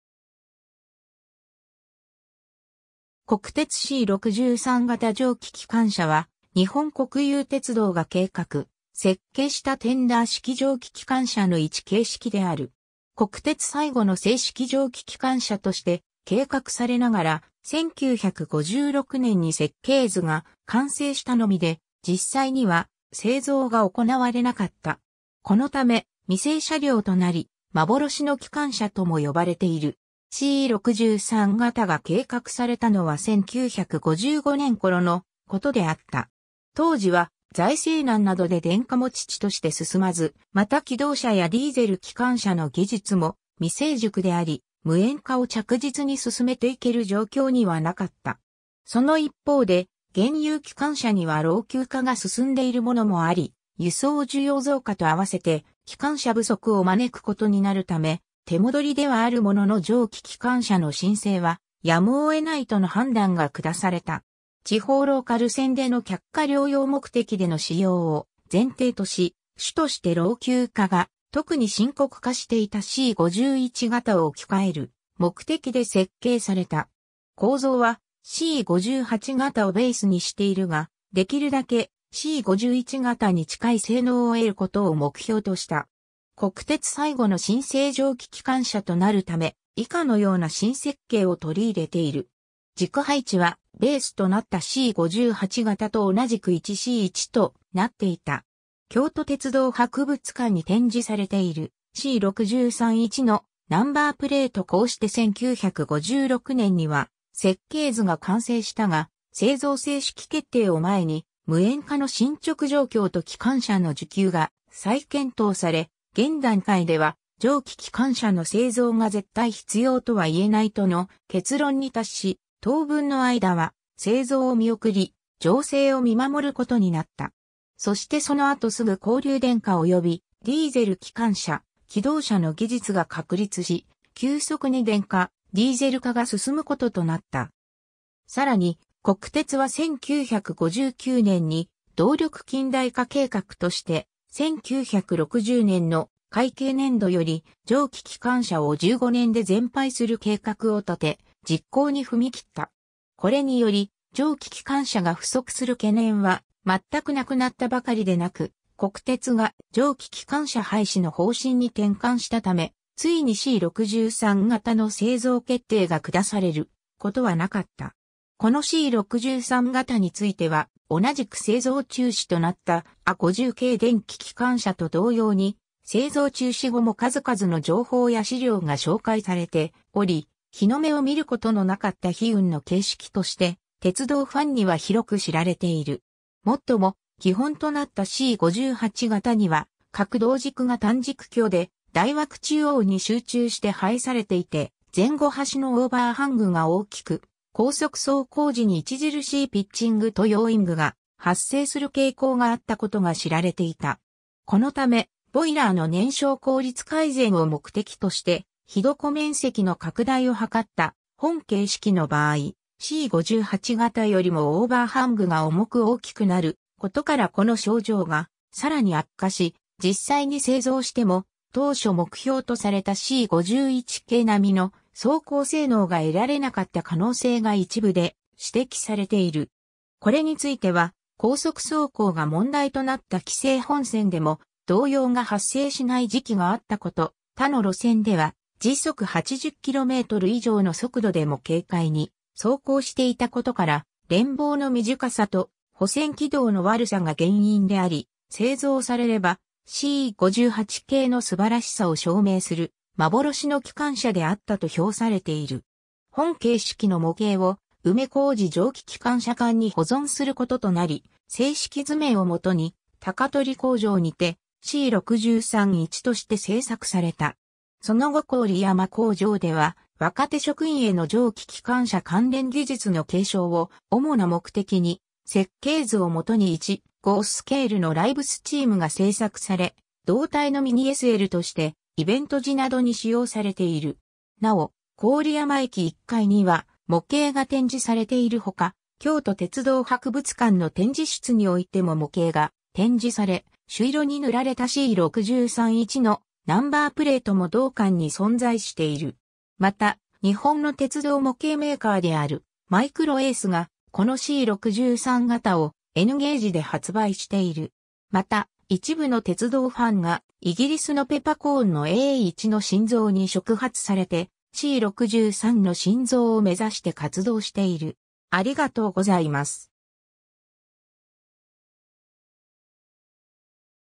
国鉄C63形蒸気機関車
車輪配置2-6-2の機関車
国鉄C63形蒸気機関車（こくてつC63がたじょうききかんしゃ）は、日本国有鉄道（国鉄）が計画・設計したテンダー式蒸気機関車の一形式である。
国鉄最後の制式蒸気機関車として計画されながら、1956年（昭和31年）に設計図が完成したのみで、実際には製造が行われなかった。
このため未成車両となり、幻の（蒸気）機関車とも呼ばれている。
 C63形が計画されたのは1955年（昭和30年）頃のことであった。
当時は財政難などで電化も遅々として進まず、また気動車やディーゼル機関車の技術も未成熟であり、無煙化を着実に進めていける状況にはなかった。
その一方で現有機関車には老朽化が進んでいるものもあり、輸送需要増加と合わせて機関車不足を招くことになるため、「手戻り」ではあるものの蒸気機関車の新製はやむを得ないとの判断が下された。
 地方ローカル線での客貨両用目的での使用を前提とし、主として老朽化が特に深刻化していたC51形を置き換える目的で設計された。
構造はC58形をベースにしているが、できるだけC51形に近い性能を得ることを目標とした。
 国鉄最後の新製蒸気機関車となるため、以下のような新設計を取り入れている。
 軸配置はベースとなったC58形と同じく1C1（ホワイト式：2-6-2、アメリカ式：プレーリー）となっていた。
 京都鉄道博物館に展示されているC63 1のナンバープレート こうして1956年（昭和31年）には設計図が完成したが、製造正式決定を前に、無煙化の進捗状況と機関車の需給が再検討され、現段階では蒸気機関車の製造が絶対必要とはいえないとの結論に達し、当分の間は製造を見送り、情勢を見守ることになった。
そしてその後すぐ交流電化及びディーゼル機関車・気動車の技術が確立し、急速に電化・ディーゼル化が進むこととなった。
 さらに、国鉄は1959年（昭和34年）に「動力近代化計画」として、1960年（昭和35年）の会計年度より蒸気機関車を15年で全廃する計画を立て、実行に踏み切った（完了は予定通り1975年度の年度末となる1976年3月）。
これにより、蒸気機関車が不足する懸念は全くなくなったばかりでなく、国鉄が蒸気機関車廃止の方針に転換したため、ついにC63形の製造決定が下されることはなかった。
 このC63形については、同じく製造中止となったEH50形電気機関車と同様に、製造中止後も数々の情報や資料が紹介されており、日の目を見ることのなかった悲運の形式として、鉄道ファンには広く知られている。
もっとも、基本となったC58形には、各動軸が短軸距で台枠中央に集中して配されていて前後端のオーバーハングが大きく、高速走行時に著しいピッチングとヨーイングが発生する傾向があったことが知られていた。
このため、ボイラーの燃焼効率改善を目的として火床面積の拡大を図った本形式の場合、C58形よりもオーバーハングが重く大きくなることからこの症状がさらに悪化し、実際に製造しても当初目標とされたC51形並の走行性能が得られなかった可能性が一部で指摘されている。
 これについては、高速走行が問題となった紀勢本線でも動揺が発生しない時期があったこと、他の路線では80km/h以上の速度でも軽快に走行していたことから連棒の短かさと保線軌道の悪さが原因であり、製造されればC58形の素晴らしさを証明する幻の機関車であったと評されている。
 本形式の模型を梅小路蒸気機関車館に保存することとなり、正式図面を基に鷹取工場にてC63 1として製作された。
その後郡山工場（現・郡山総合車両センター）では、若手職員への蒸気機関車関連技術の継承を主な目的に、設計図を元に1/5スケールのライブスチームが製作され、動態のミニSLとしてイベント時などに使用されている。
 なお、郡山駅1階には模型が展示されている他、京都鉄道博物館の展示室においても模型が展示され、朱色に塗られたC63 1のナンバープレートも同館に存在している。
また、日本の鉄道模型メーカーであるマイクロエースがこのC63形をNゲージで発売している。
また、一部の鉄道ファンが、イギリスのぺパコーンのA1の新造に触発されてC63の新造を目指して活動している。